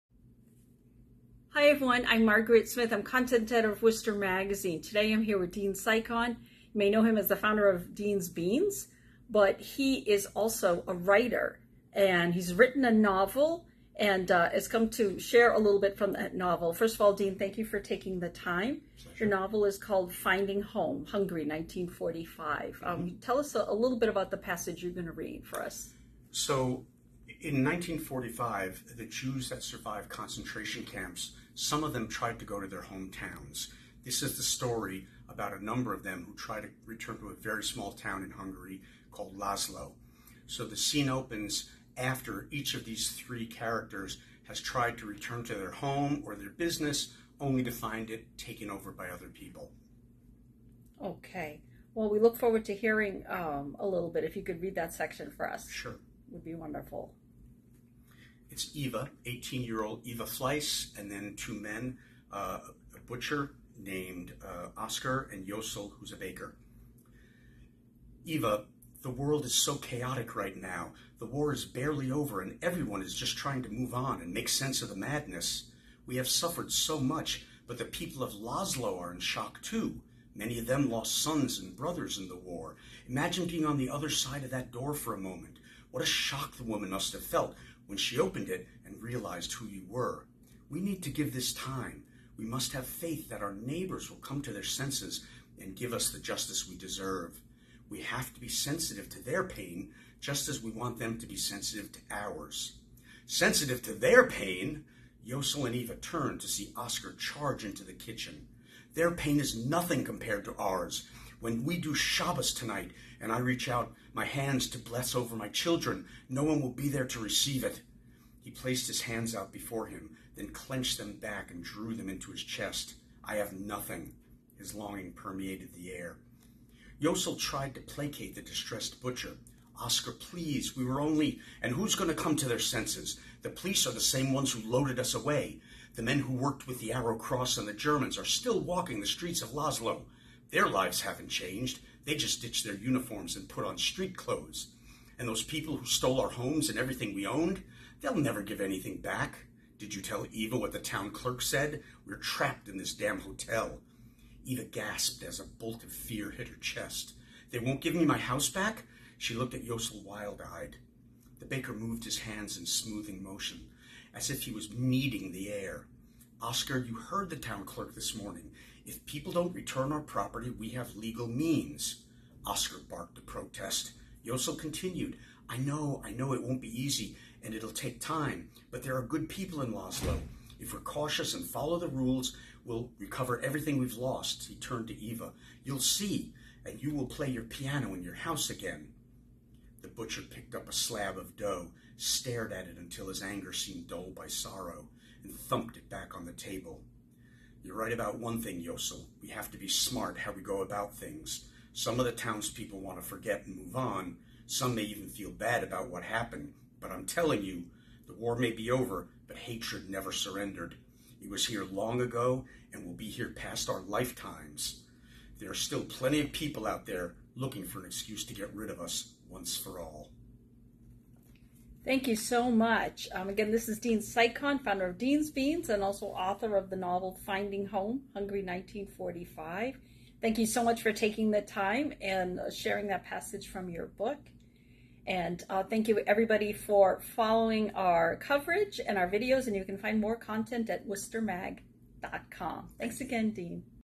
Dean Cycon reads from his novel, "Finding Home (Hungary 1945")
Dean Cycon, founder of Dean's Beans Organic Coffee Company, reads from his novel, "Finding Home (Hungary 1945"), inspired by concentration camp survivors who returned to find their property taken over.
Read the complete interview at worcestermag.com.
Video and interview by Margaret Smith
Recorded at the Worcester Magazine office, July 26, 2023.
Copright 2023 Worcester Magazine/Gannett New England